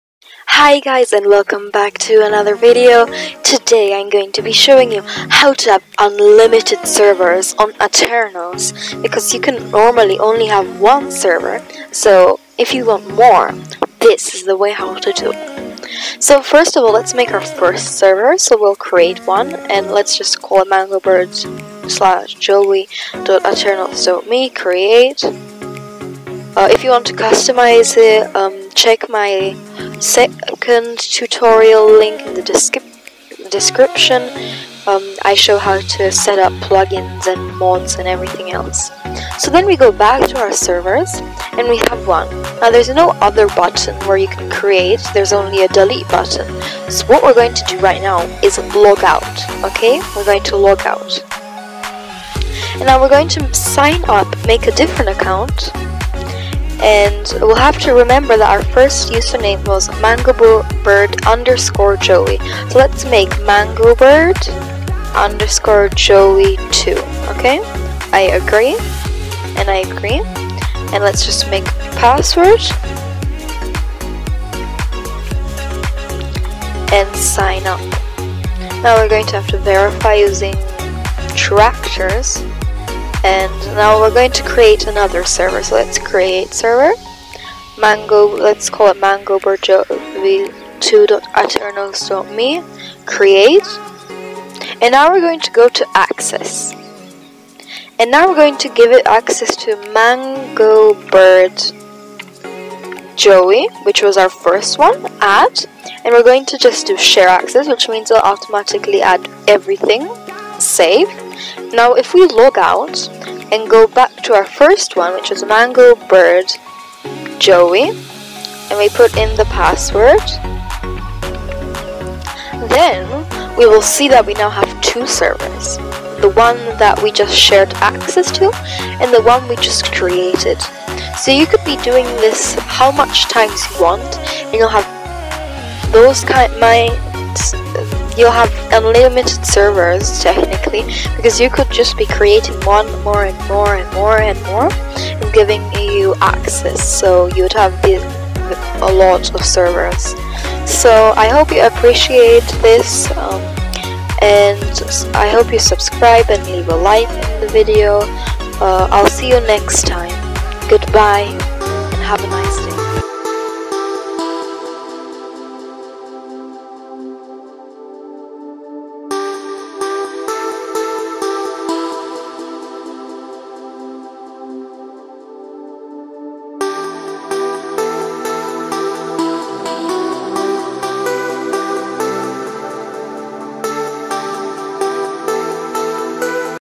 How to have UNLIMITED FREE Minecraft Servers on Aternos
Have you ever wondered how to have an infinite amount of free servers that you can manage on Aternos? Well, this really quick and easy tutorial shows you how! All you need to do is go to aternos and create a profile and server, but you can only have one server per profile? So how do you get unlimited servers? Watch this easy tutorial to learn the exploit.